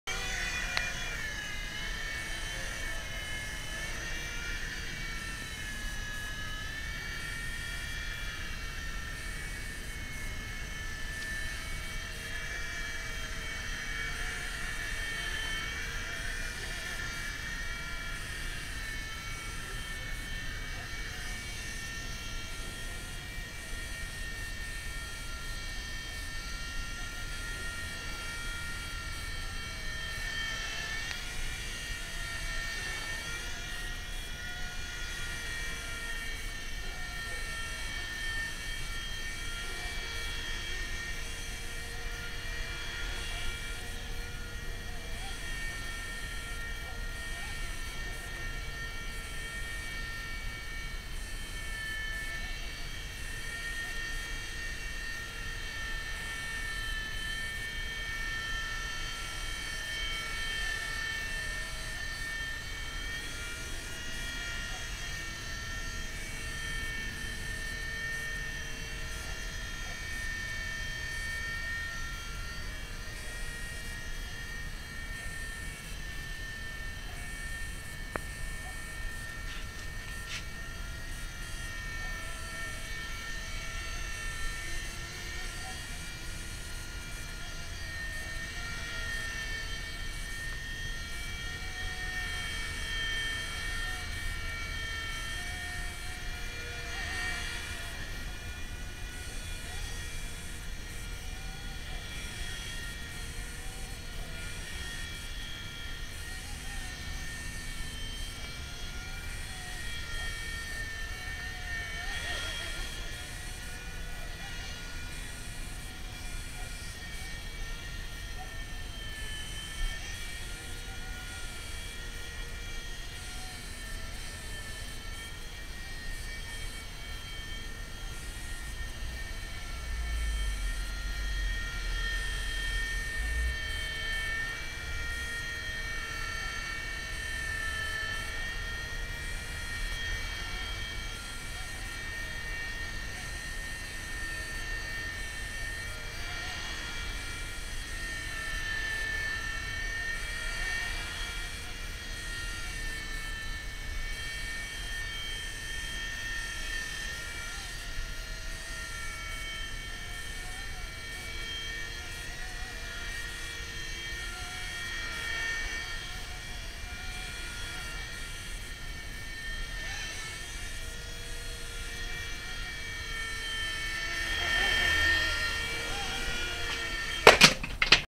wl toys v911 outdoor hovering practice (from banggood.com)
Calm winds with occasional light breeze.